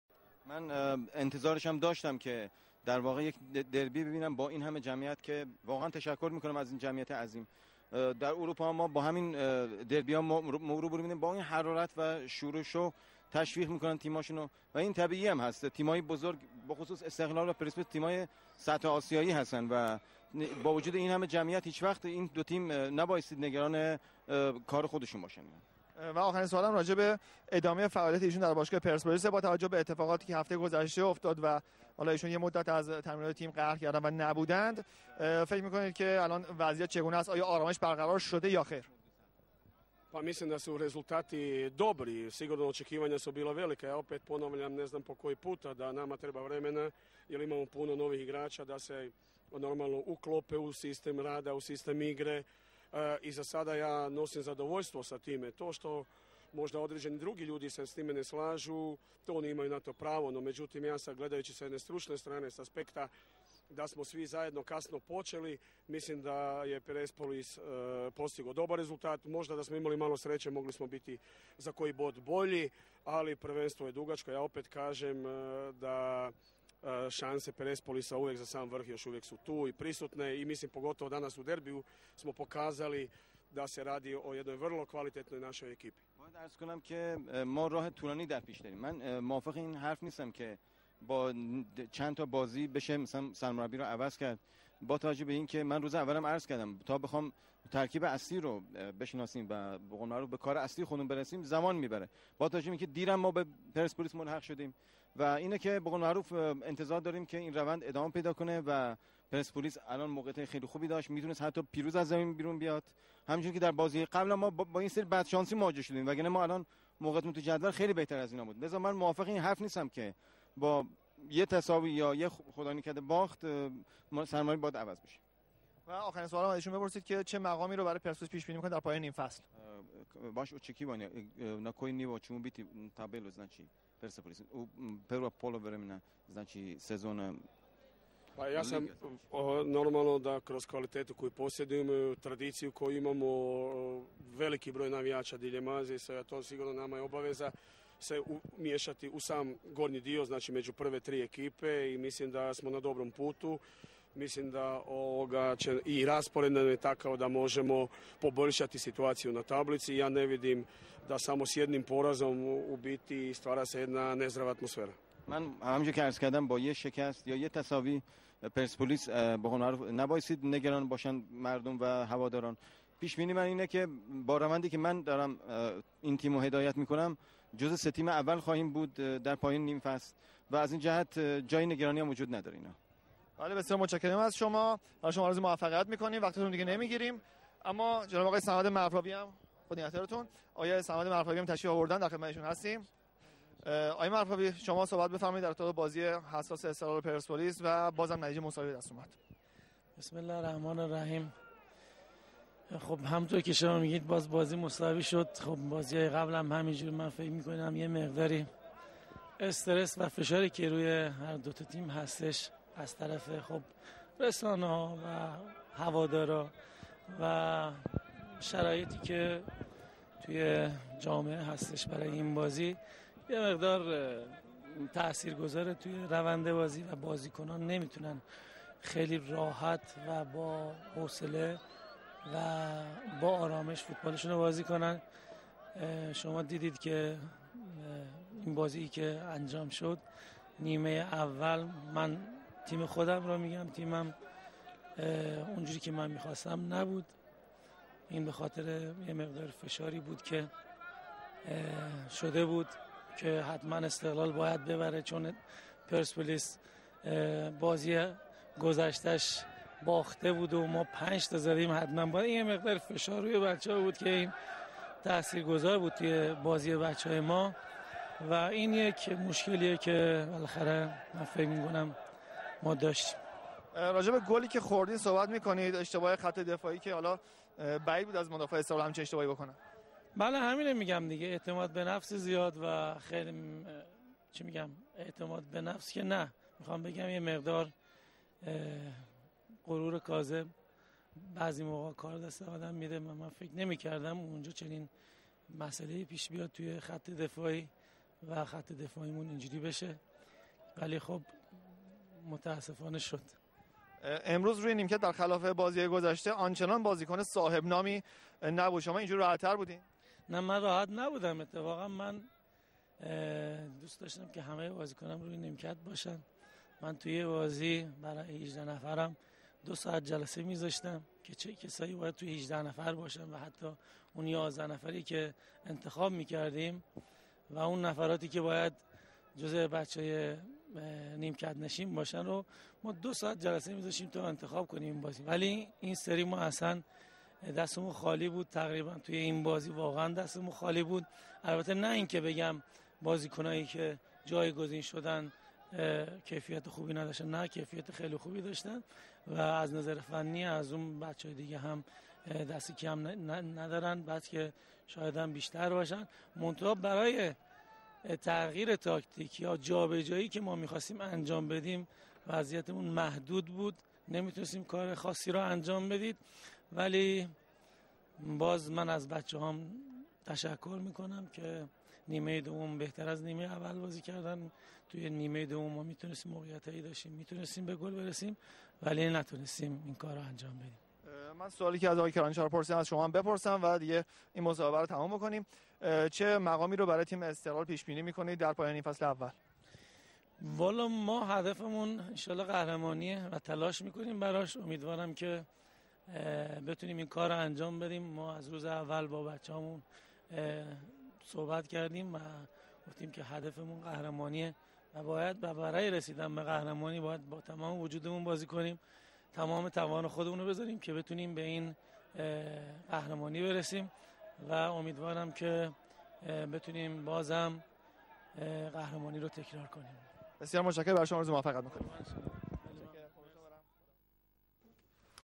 Interview with Kranjar and Marfavi after Esteghlal Perspolis derby 67
Direct interview with Perspolis coach Zlatko Kranjcar and Samad Marfavi Esteghlal's coach after the game which ended 1-1.